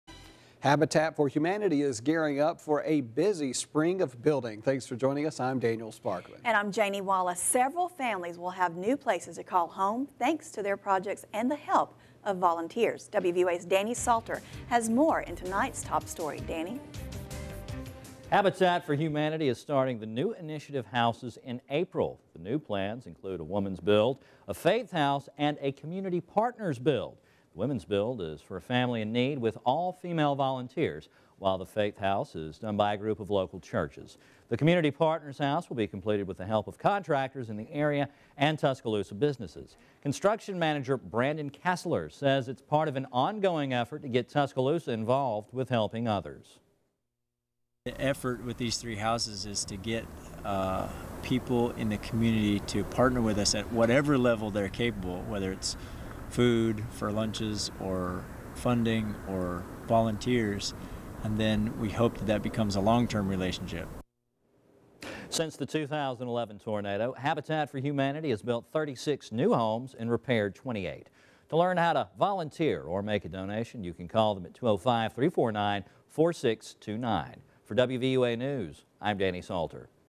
Habitat for Humanity - Tuscaloosa
Habitat for humanity is gearing up for a busy spring of building. Several families will have new places to call home thanks to their projects and the help of volunteers. Habitat for humanity is starting the new initiative houses in april.the new plans include a women's build, a faith house, and a community partners build. The women's build is for a family in need with all female volunteers, while the faith house is done by a group of local churches. The community partners house will be completed with the help of contractors in the area and tuscaloosa businesses. Construction manager brandon kasteler says it's part of an ongoing effort to get tuscaloosa involved with helping others. Since the 20-11 tornado, habitat for humanity has built 36 new homes and repaired 28. To learn how to volunteer or make a donation, you can call them at (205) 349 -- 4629.

OAD: Friday, February 7th, 2014